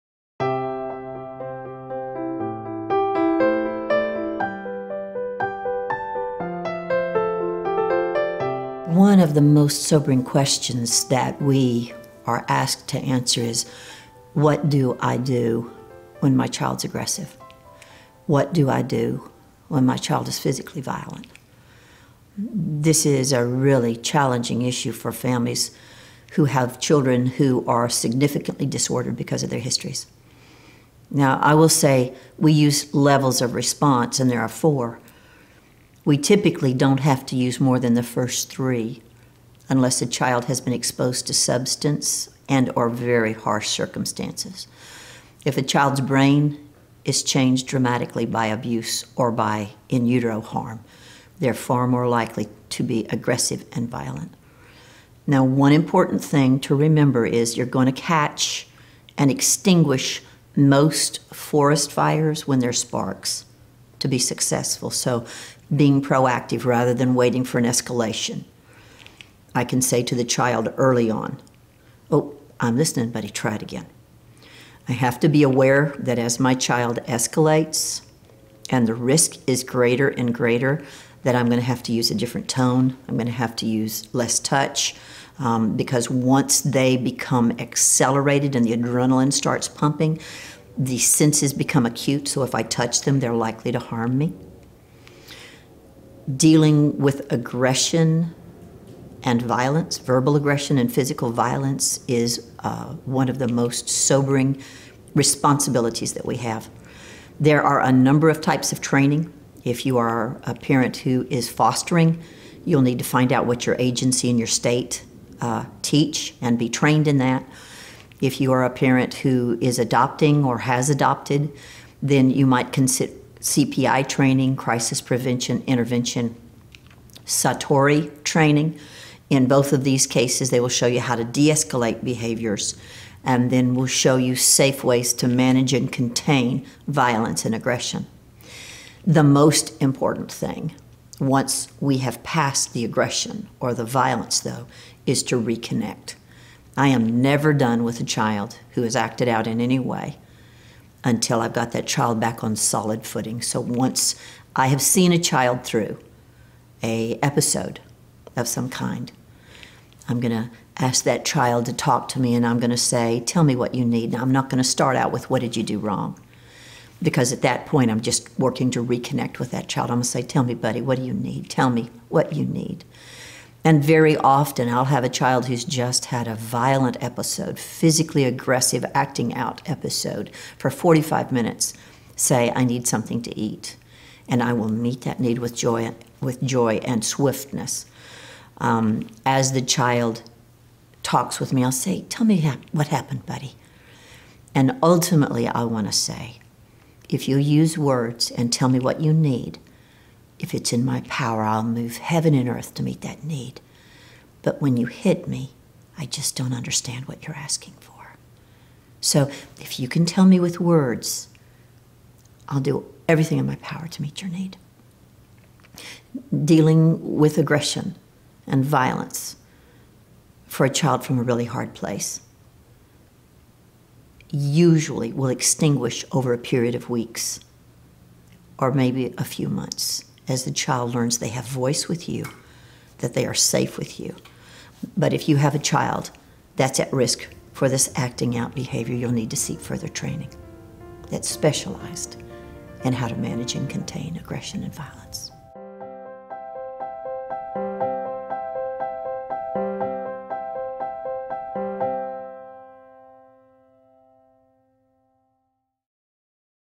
Responding to Aggressive and Violent Behavior
Watch as Dr. Karyn Purvis provides wisdom and insight for adoptive and foster parents that need help responding to aggressive and violent behaviors from a child.